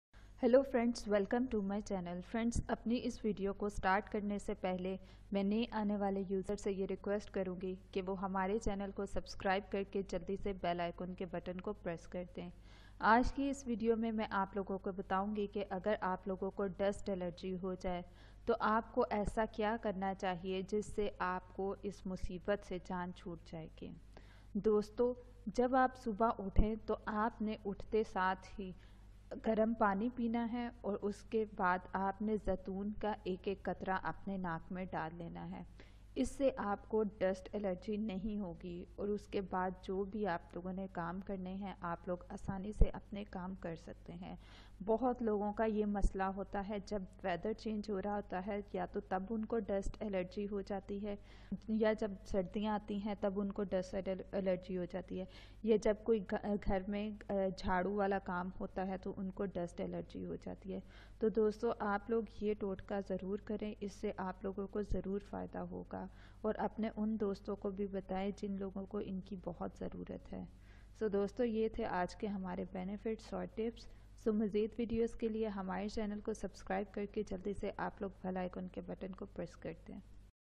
Dust Allergy Ka Jar Se Khatma aur Gharelo Totka | Natural Dust Allergy Treatment in Urdu and Hindi
Aj ki is video ma mein ap ko btao gi agar ap logo ko dust allergy hu jae tou ap ko asa kia karna chahiye jis se ap ko is musibat se jaan chut jae gi. Jab ap subha uthay tou ap ny uthtay seth hi garam pani pena hai aur us k bad ap ny zaitoon ka ek ek katra ap ny apny naak main dal lena hai is se ap ko dust allergy nhi hu gi aur jo bhi ap ny kaam karna hai ap log asani se apny kaam kar saktay hai bohut logo ka ya masla huta hai jb weather change hu raha huta hai ya tou tab un ko dust allergy hu jati hai ya jab sardia ati hai tab un ko dust allergy hu jati hai ya jab ghar main jharoo wala kam huta hai tou un ko dust allery hu jati hai tou ap log ya gharelo totka zror kare is se ap ki allery ka ilaj hu jae ga.